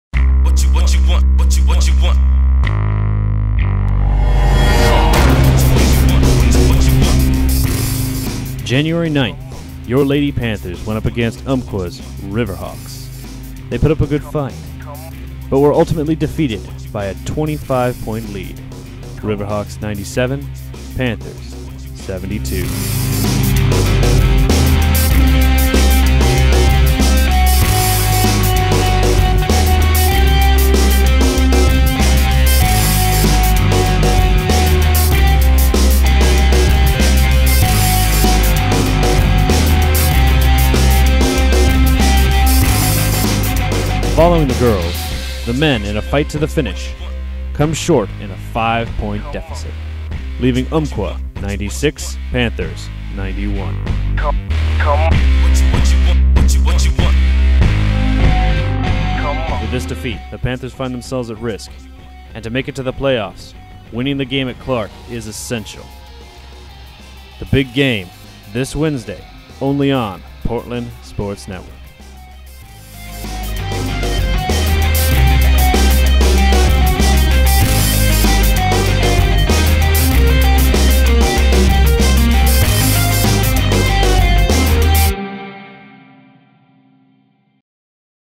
Panther's vs River Halks Game Highlight
Highlight of tonight's game PCC vs Umpqua.